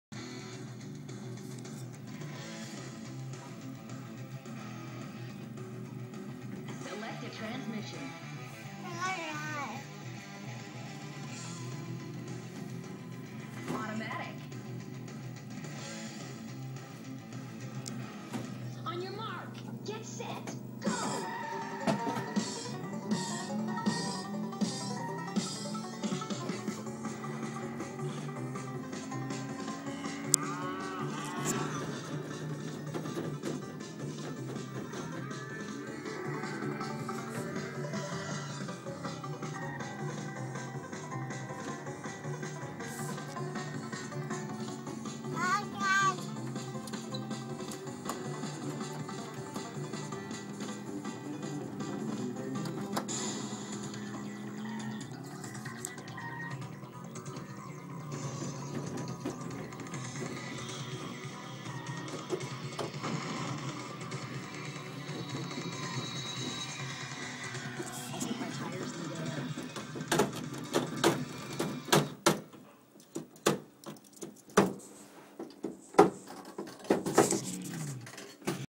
Anderson driving
Anderson driving on a video game.  He needs some training